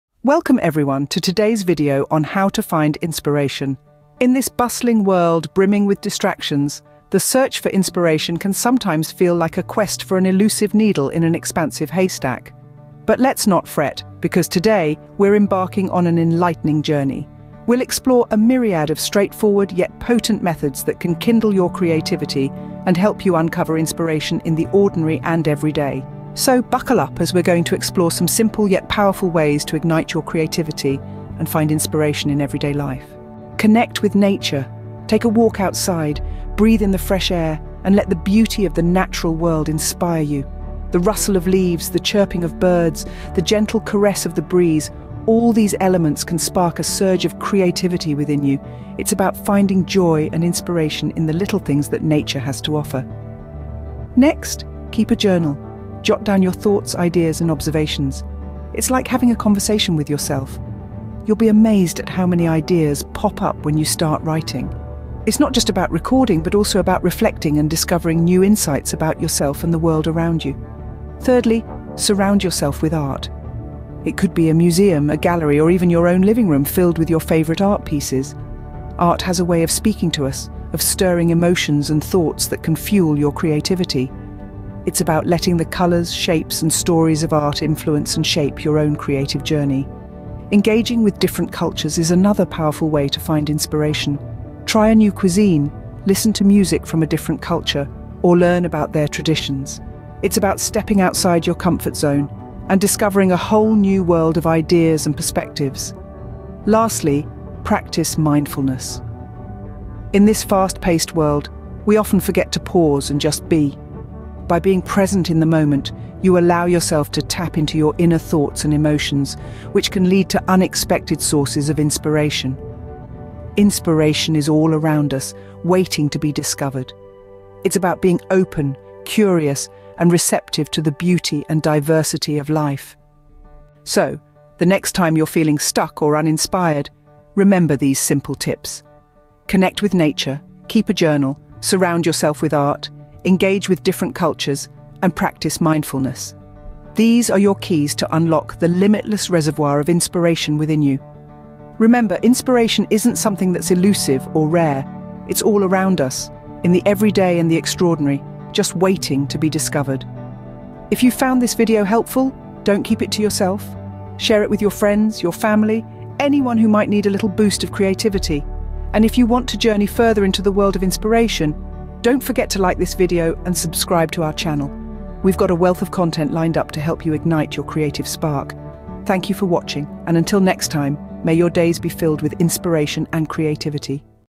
Ignite Your Creativity: How to Find Inspiration.
Uncover the secrets to finding inspiration in everyday life with our video "How to Find Inspirations." Join us as we explore simple yet powerful ways to ignite your creativity, from connecting with nature to practicing mindfulness. Discover how to tap into your inner wellspring of inspiration and unleash your creative potential